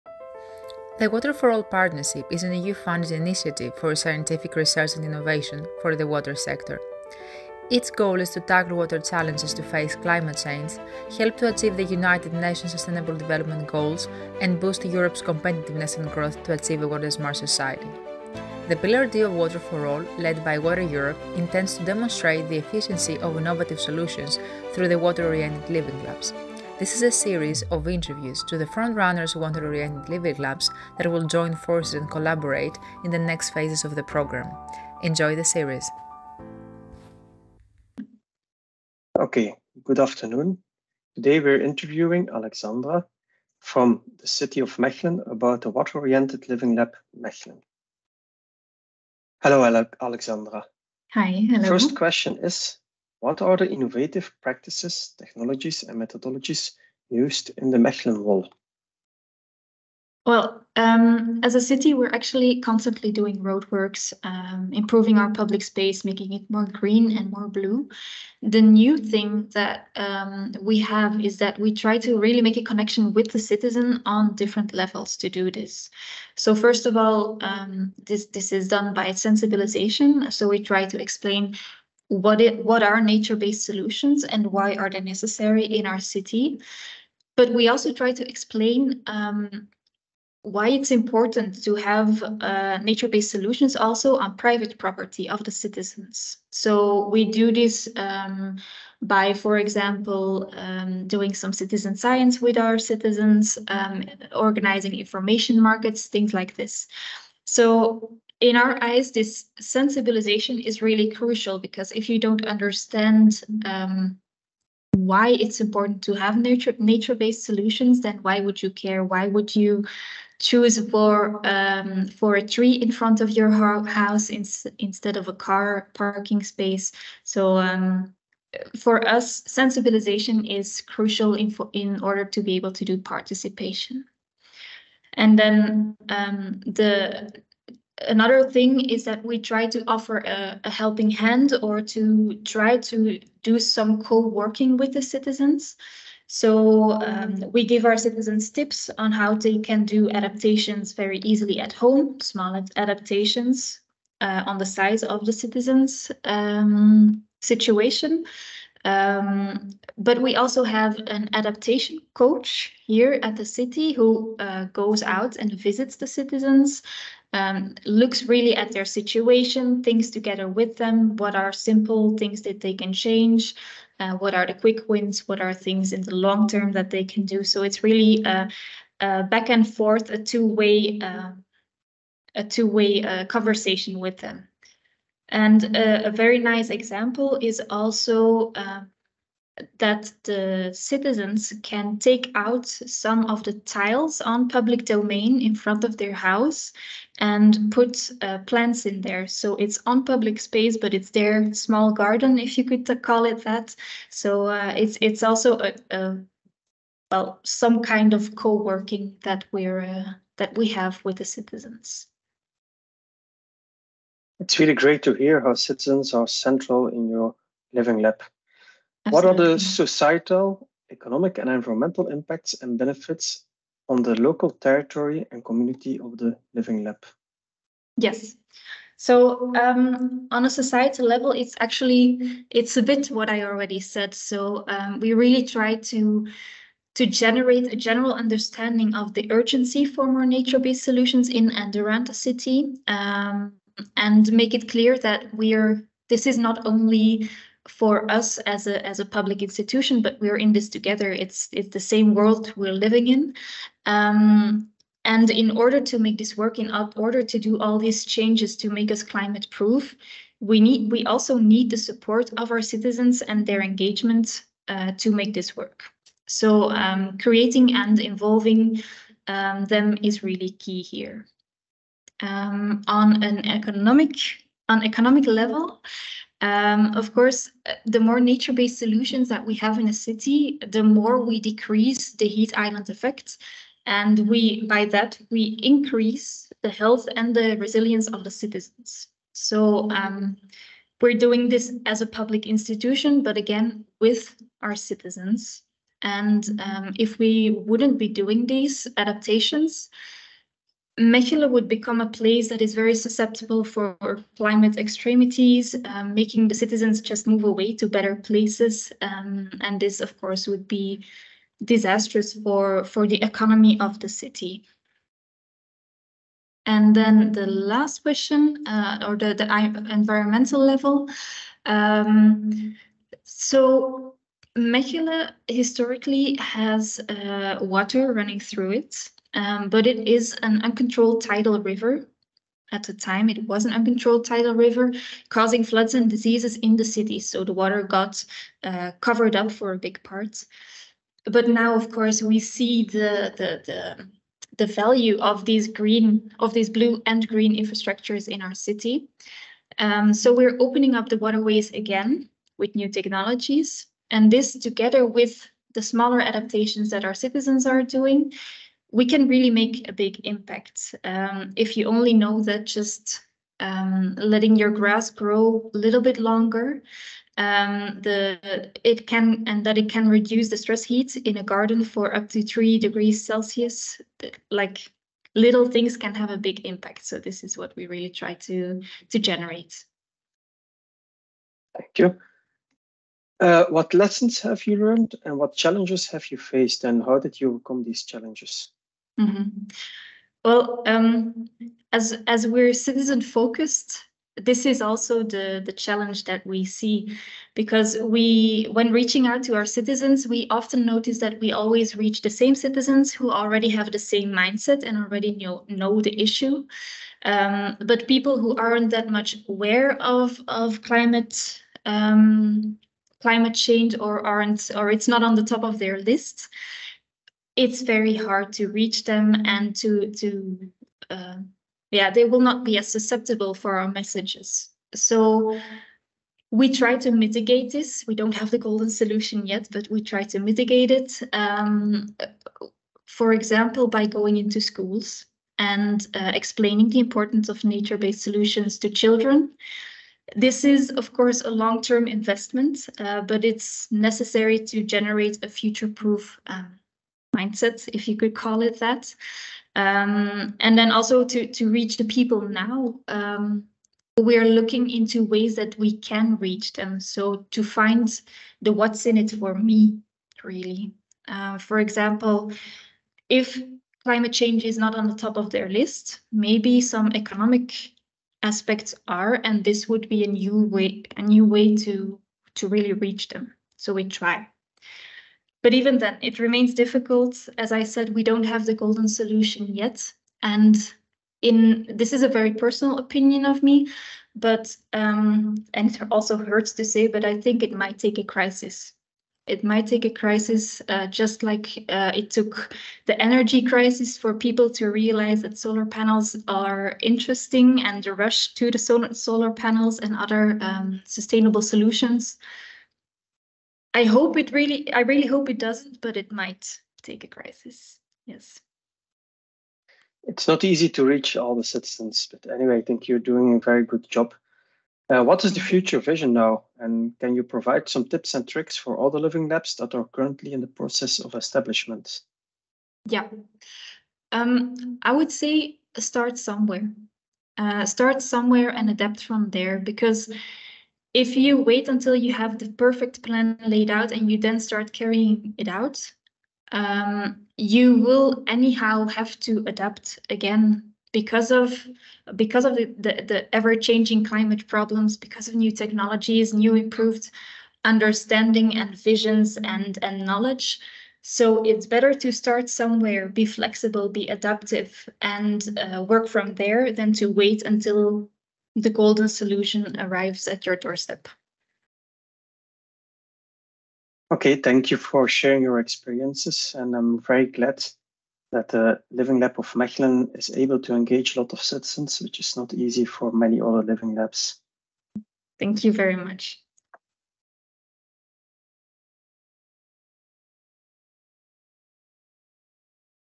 Mechelen’s Path to Sustainability: Engaging Citizens in Climate Adaptation
In Mechelen, the push for a climate-resilient future is a collaborative effort between the city and its residents.

Through participatory roadworks, depaving projects, and reopening old water channels, the city aims to adapt its urban infrastructure to climate challenges.

Citizens are actively involved in initiatives like rainwater management, greening gardens, and green roofs. With creative activities such as tile gardens and depaving championships, Mechelen fosters community engagement and promotes nature-based solutions.

This approach blends top-down and bottom-up efforts, creating a shared vision for a sustainable, climate-resilient city._________________